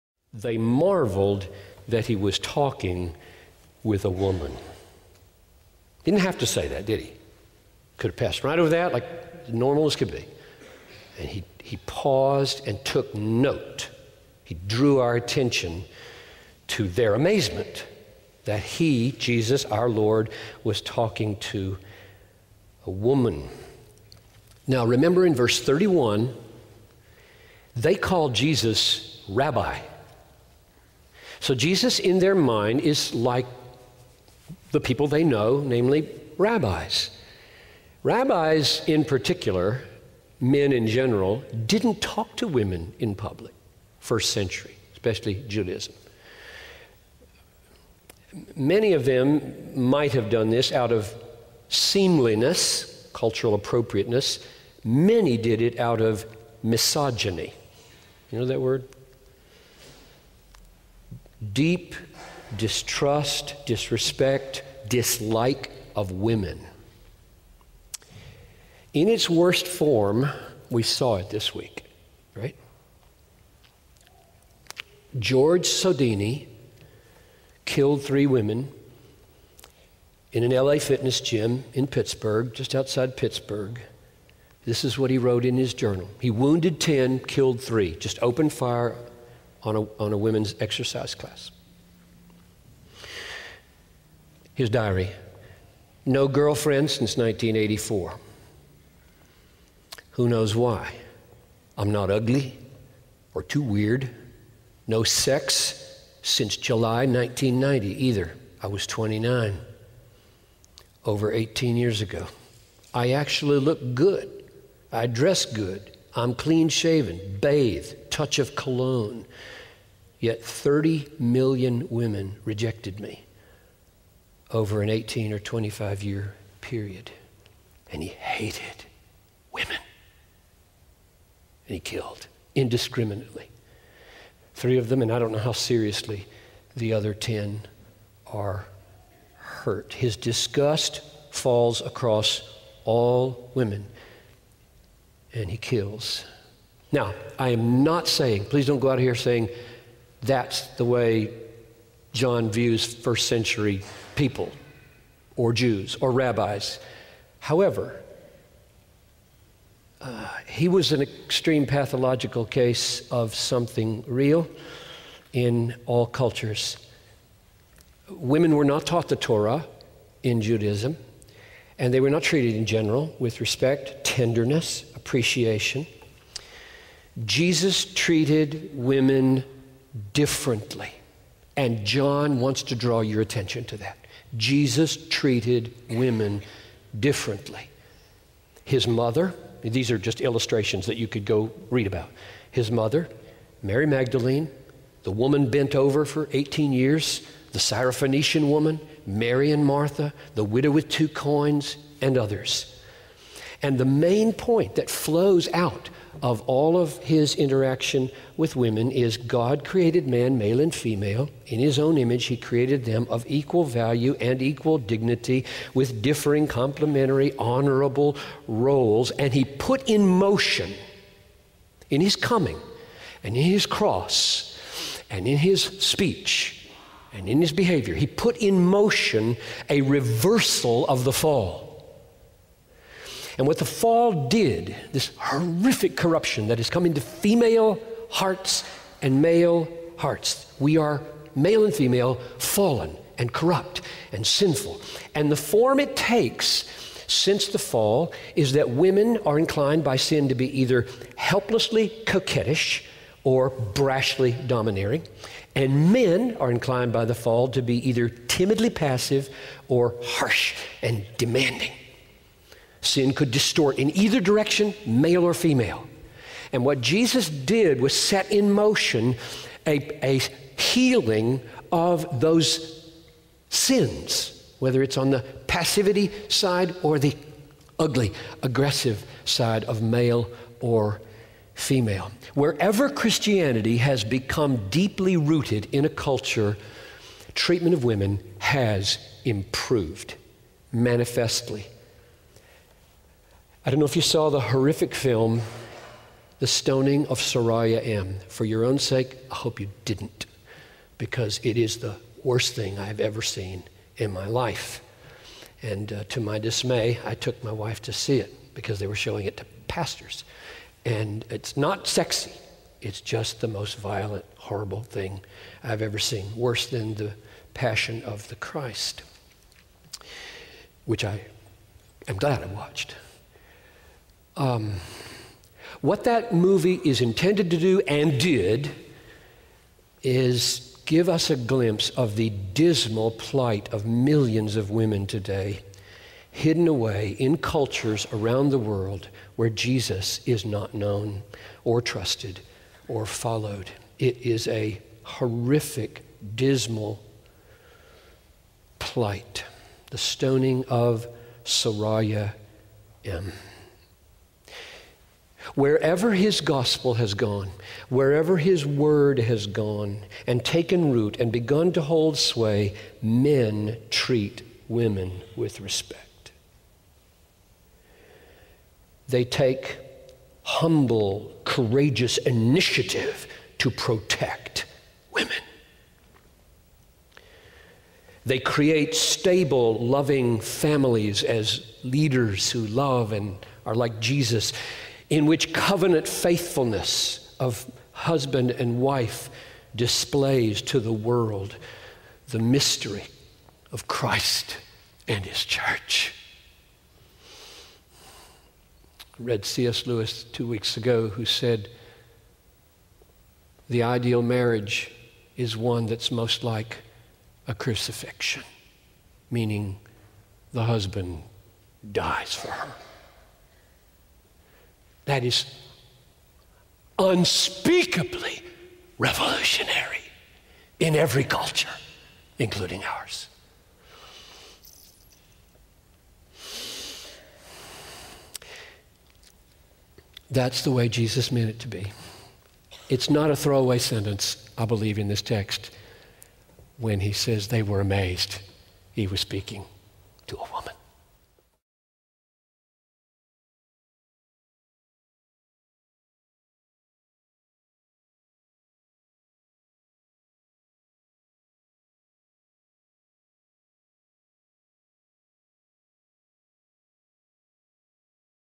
John Piper - Jesús trató a las mujeres de una manera diferente
"The Food of Christ Is to Give Eternal Life"
John 4:27-42